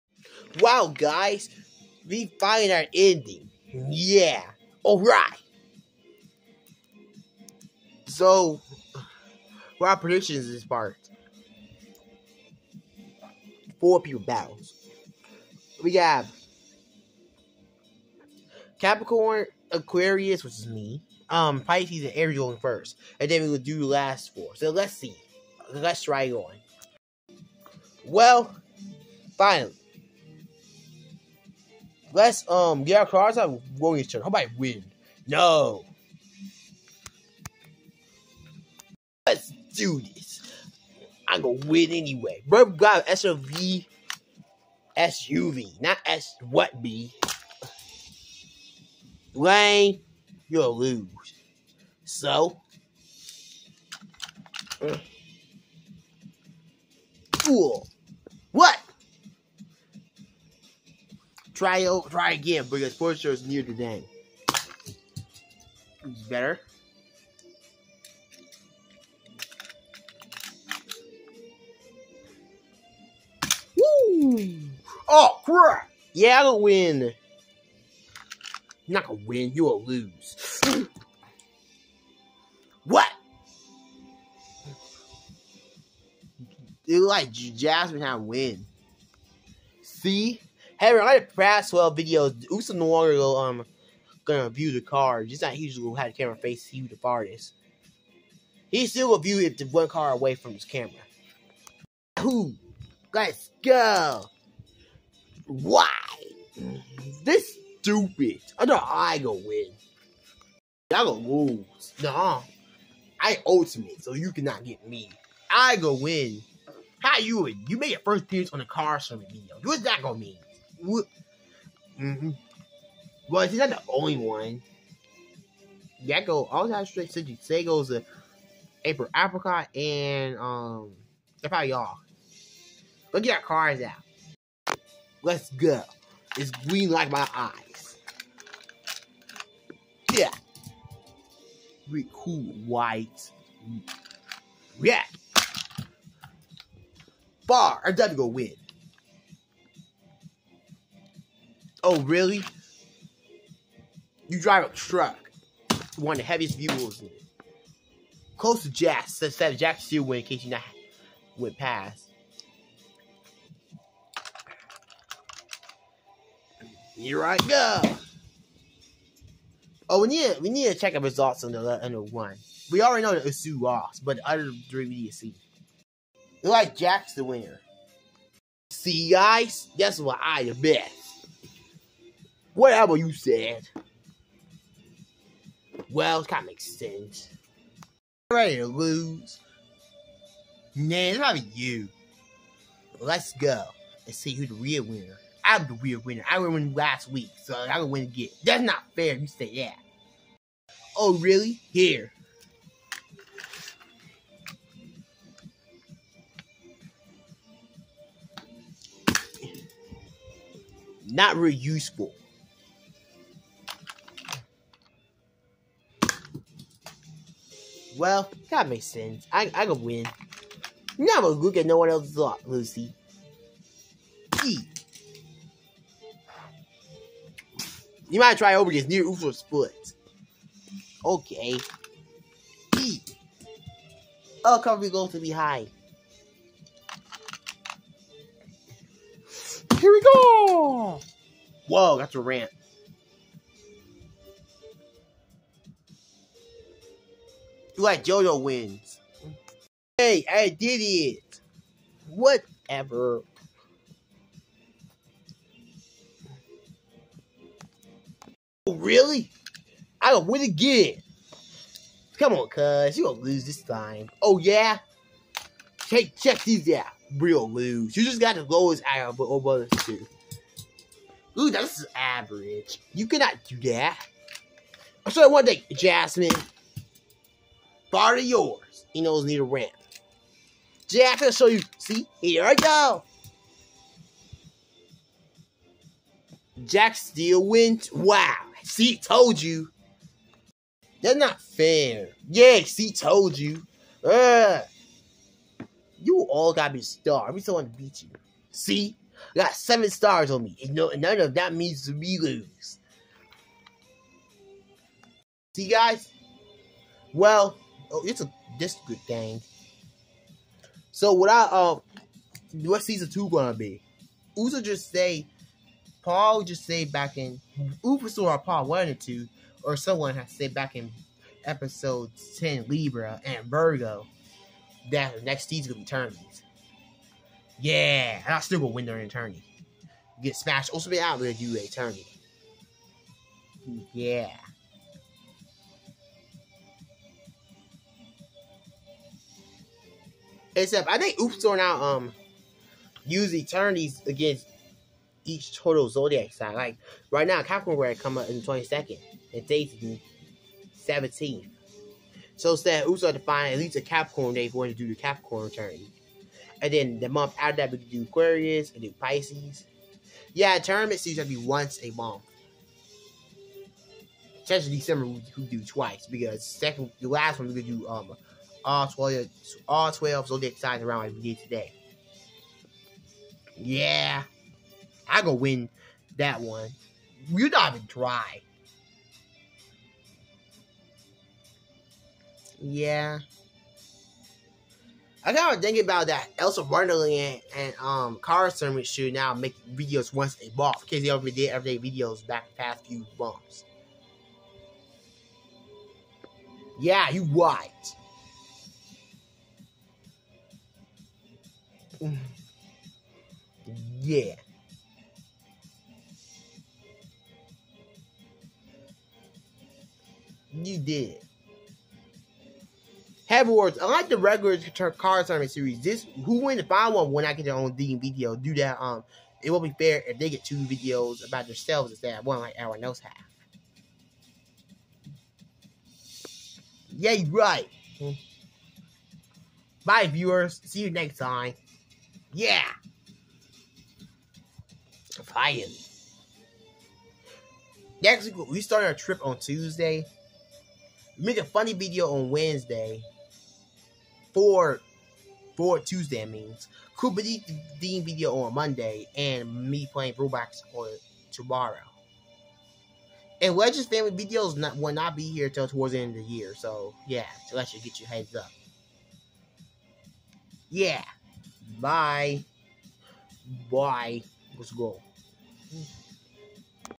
Cars Tournament Episode 13 Finale: All 12 Zodiac Signs Battle
Characters: Gecko, Jasmane, Mickey mouse, April, Asui, Jax, Clessy, Peach, Princess Strawberry, Lucy, Jojo and Pikachu